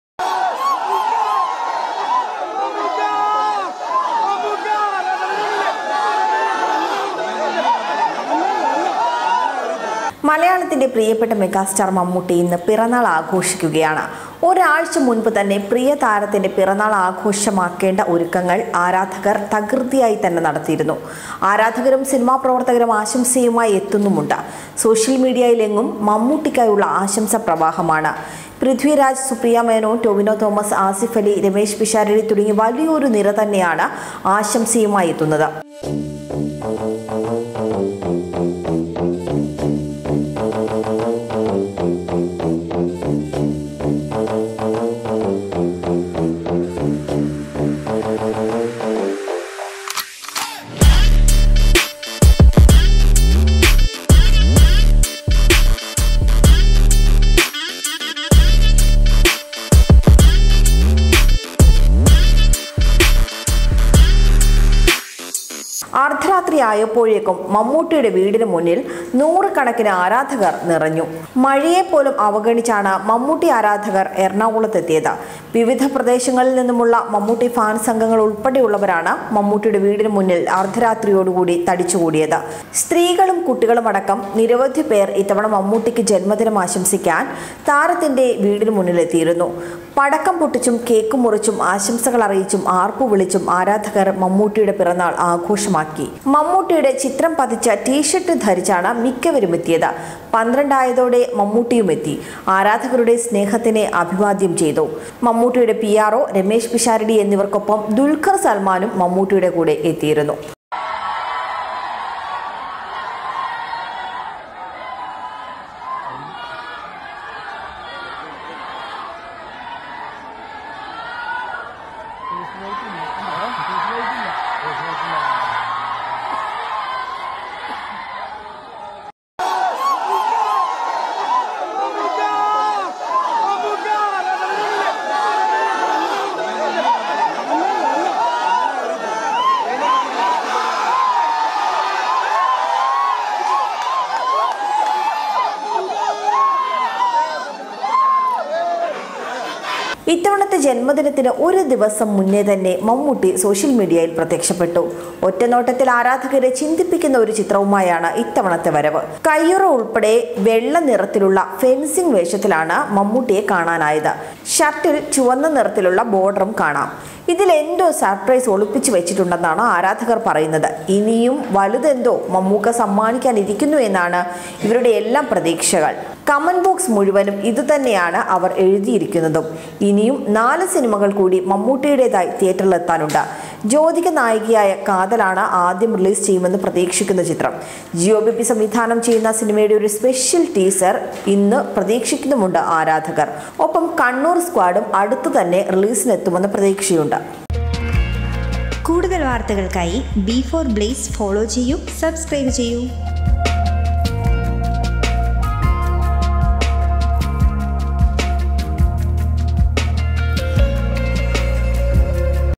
HBD Mammootty | ആർപ്പുവിളികളും, ആശംസകളുമായി ആരാധകർ; സോഷ്യൽ മീഡിയയിലും ഒരേയൊരു മമ്മൂട്ടി
ഒരാഴ്ച മുൻപ് തന്നെ പ്രിയ താരത്തിന്റെ പിറന്നാൾ ആഘോഷമാക്കേണ്ട ഒരുക്കങ്ങൾ ആരാധകർ തകൃതിയായി നടത്തിയിരുന്നു. ആരാധകരും സിനിമാ പ്രവർത്തകരും ആശംസയുമായി എത്തിയിട്ടുണ്ട്. സോഷ്യൽ മീഡിയയിലെങ്ങും മമ്മൂട്ടിക്കായുള്ള ആശംസാ പ്രവാഹമാണ്.